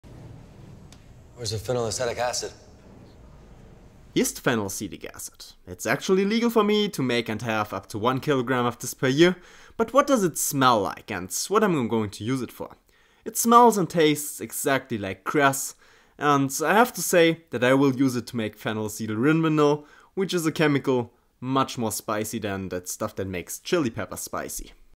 Phenylacetic Acid
This is phenylacetic acid which has been talked about occasionally in the series breakingbad but which most of you have never seen before. I am allowed to make/own up to 1kg of this substance every year and because I have legitimate, legal uses for it I can make it without any problem. The phenylacetic acid will be used by me to make phenylacetylrinvanil which is 300x as spicy as pure capsaicin, penicillin which is the first antibiotic ever created and a few other interesting things like esters which have a great smell.